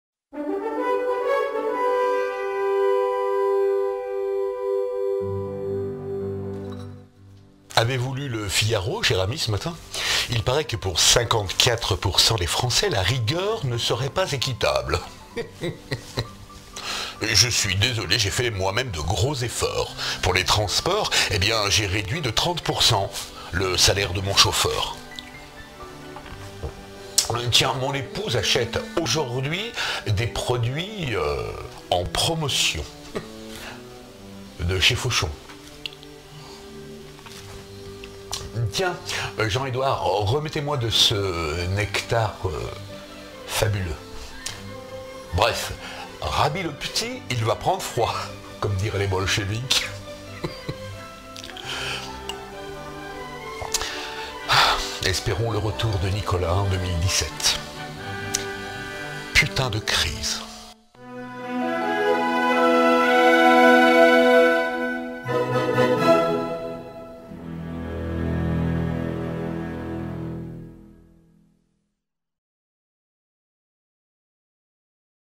Brève de comptoir au Fouquet's - La rigueur est équitable !
54% des français pensent que la rigueur qui nous ait imposés n'est pas équitable (source de l'info)  Un sujet qui fait causer dans "certains" bistros !!